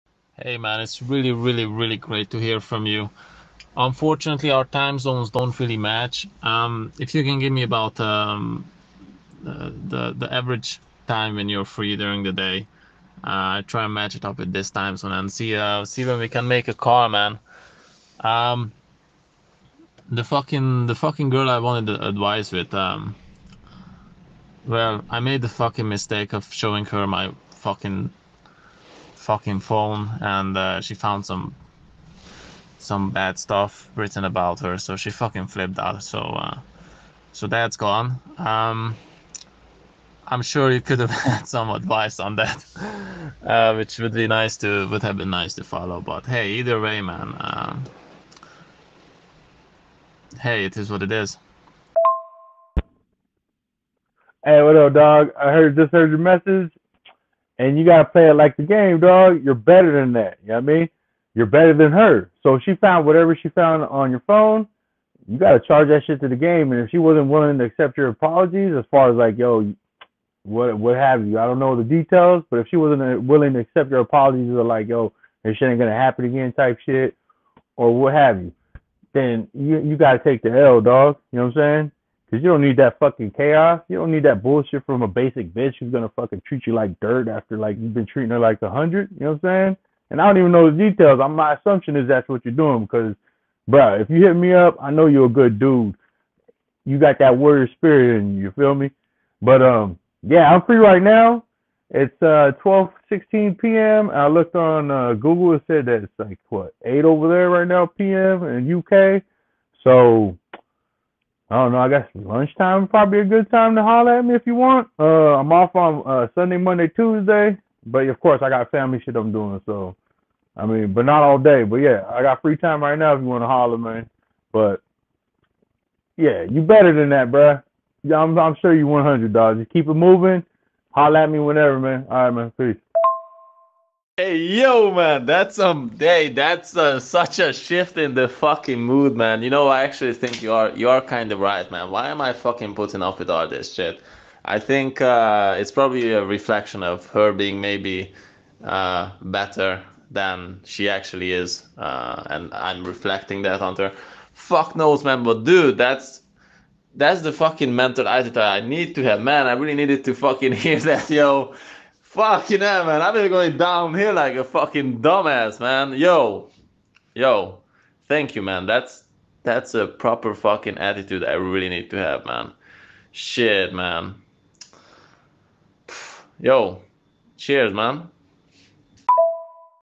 GMTNS- Being a Brothers Keeper Vlog 3
Being a Brothers Keeper
What is a good man but a bad man’s teacher
What is a bad man but a good man’s job
-Lao Tzu
Tribe shit

Fam from the UK needed help & this is the info that helped him get a different perspective to get himself out of his emotional chaos(heart break of chaos)

Our phone call details were:
Seeing the lady for 3 months
Dropped $5g’s on her during their time & when he felt more feeling for her he decided to let her see his phone.
Thinking it would prove to her that he is willing to be committed since he is willing to let her see the phone.

The convo about his girl was petty. Fam didn’t even call her bad names. He just was venting to his homies on her inconsistency on life choices that would’ve benefited her & him in the long run. Anyway this is our convo.
If any fam needs help holla if needed.
One love